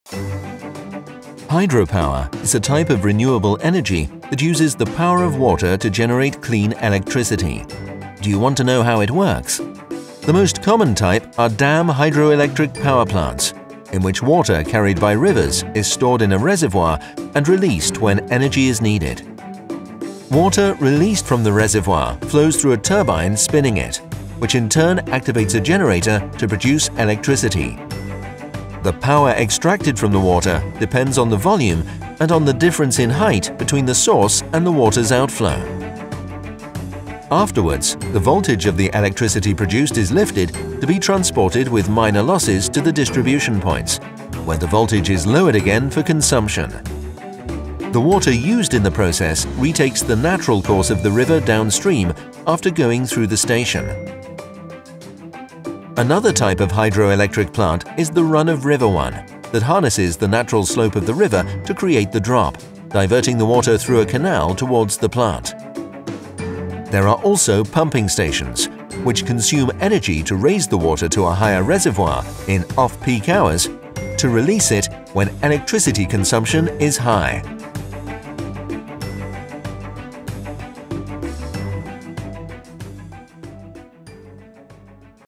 How does hydropower work?
Hydropower harnesses the power of water to generate electricity. Hydropower has existed for centuries, and there are different types of hydroelectric plants. Do you want to know how many types there are and how they work?